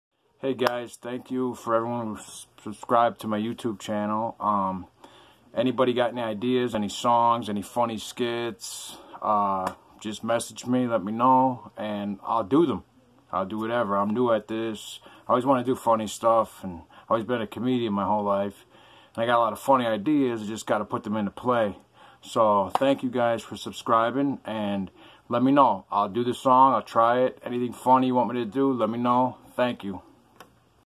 Me thanking my YouTube subscribers. And asking people for funny ideas are songs they want me to do.
Asking for ideas